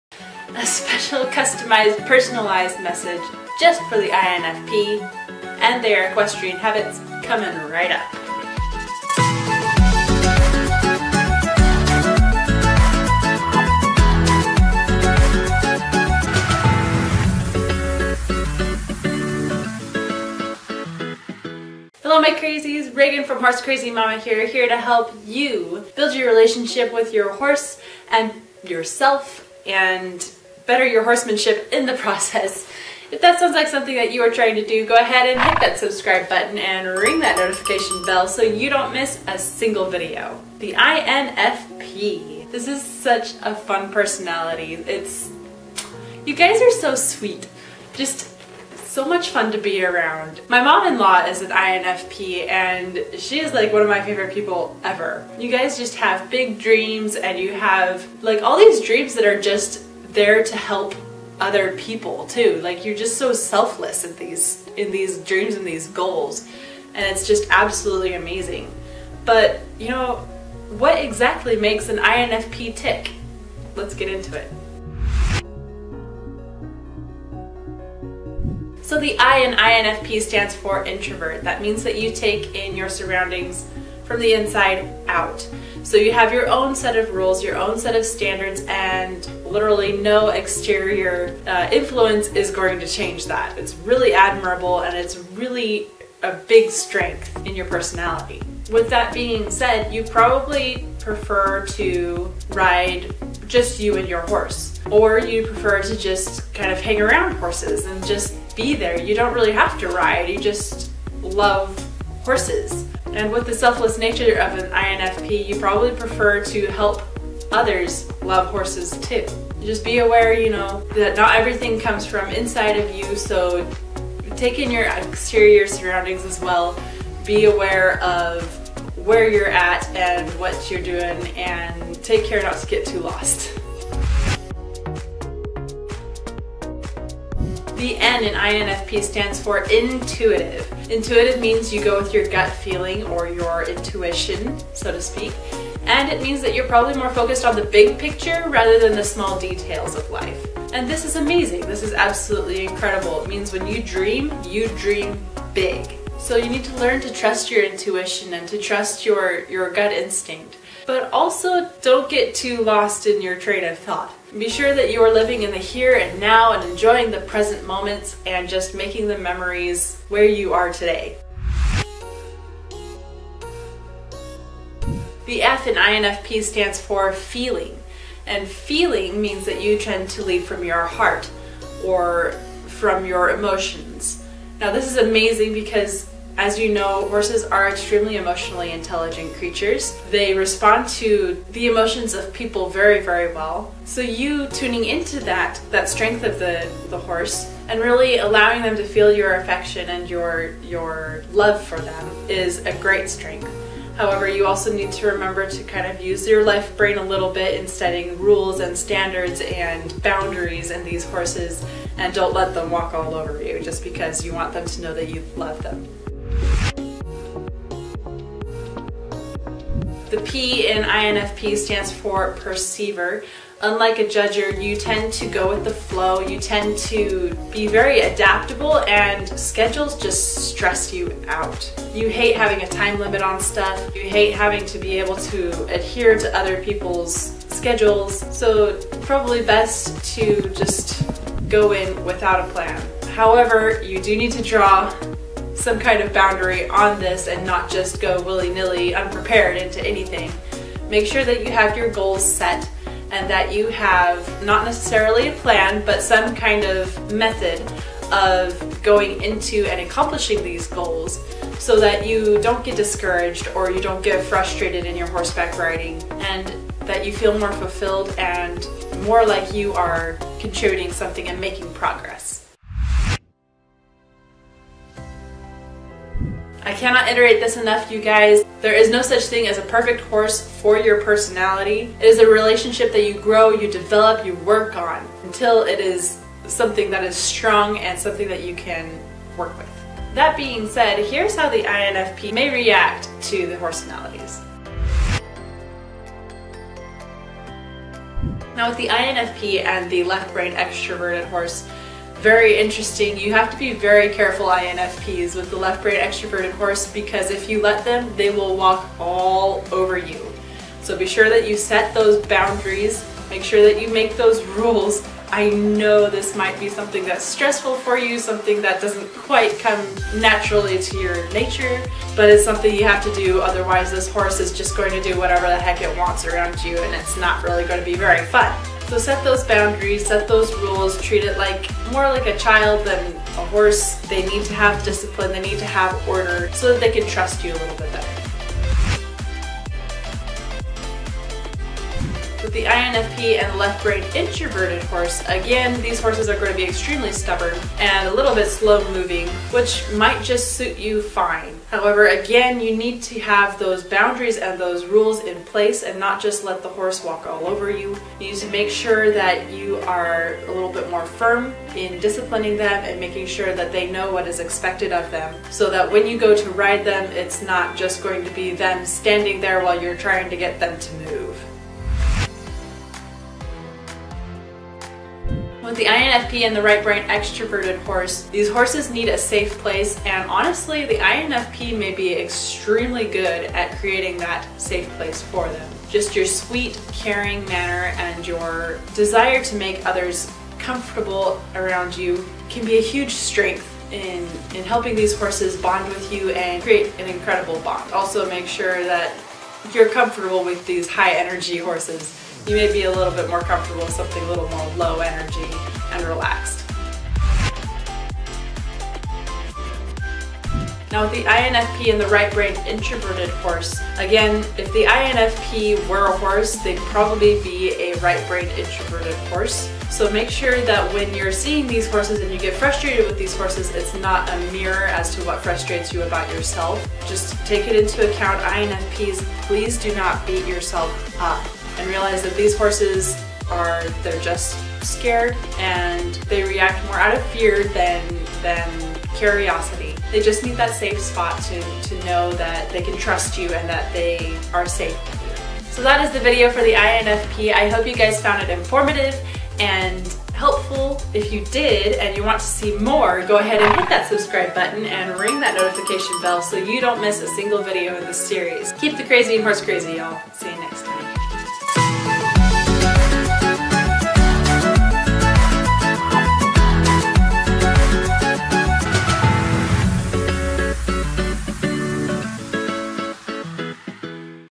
The INFP - The Tennessee Walker Personality
Work on a horse, that horse gets better. Work on yourself, all your horses get better. Here I explore the ENTJ personality from the MBTI or Meyers-Briggs Type Indicator and how they might react to Pat Parelli's Four Horsenalities. Self-awareness is key to bettering your relationship and performance with your horse.

Want To Be Featured? Submit Your Video Here!